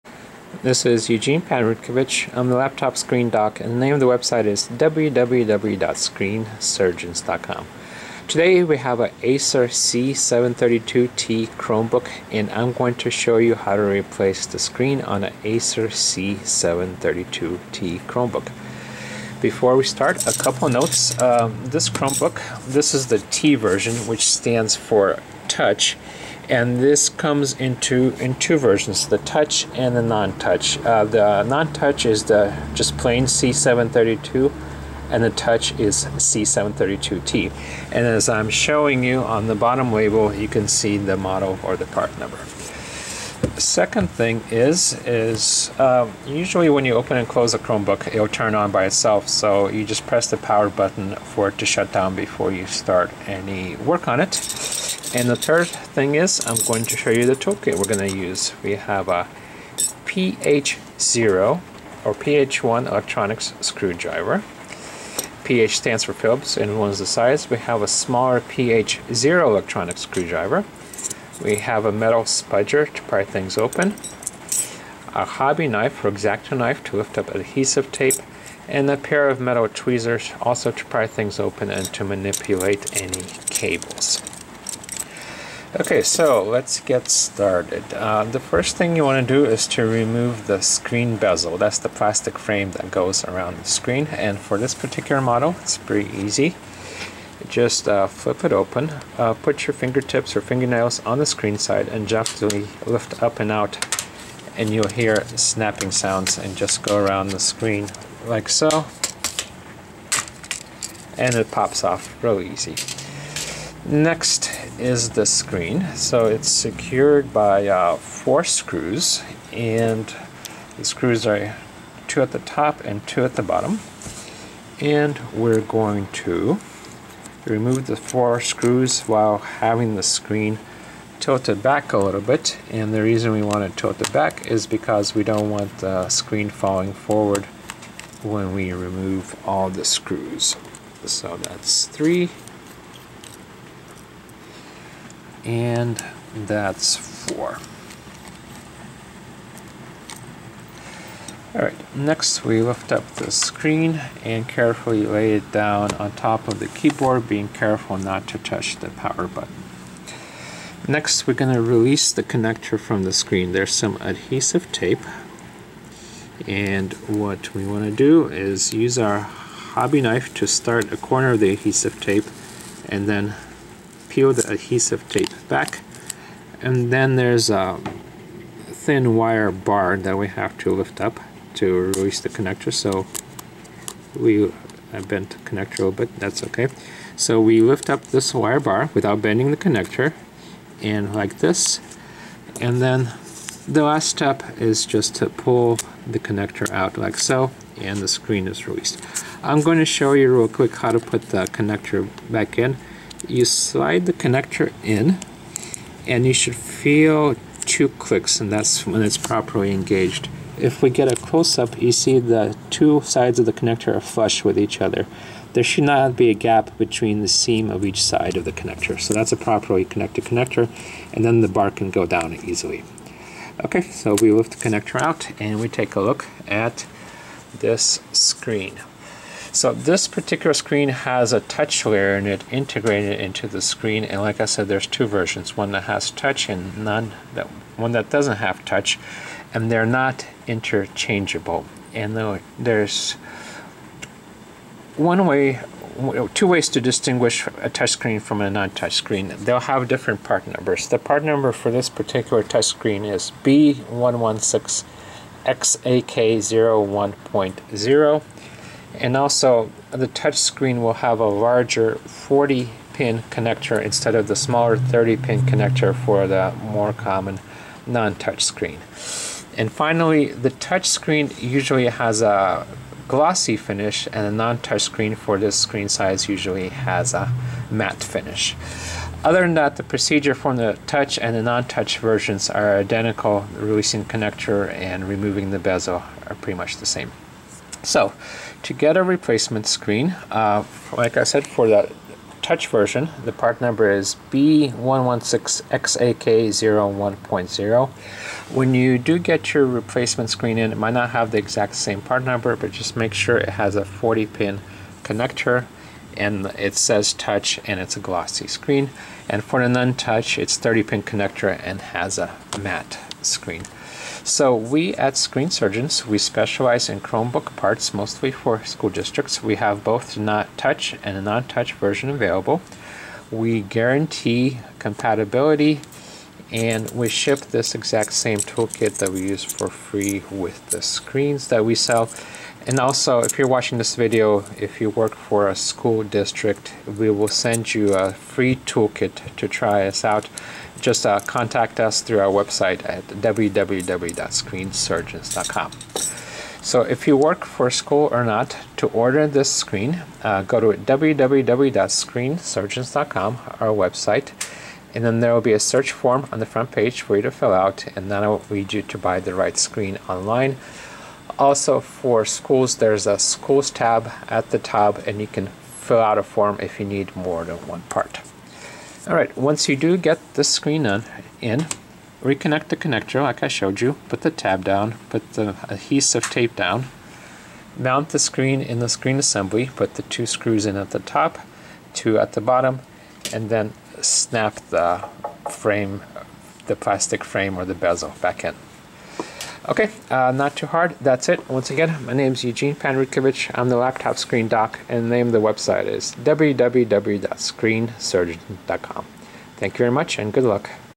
Acer Chromebook C732 C732T Screen Replacement Procedure
to find these screens.
Step by step instructions to replace a cracked screen or touch screen on a Acer C732 C732t Chromebook.
ACER CHROMEBOOK C732 REPLACEMENT LCD SCREEN (NON TOUCH)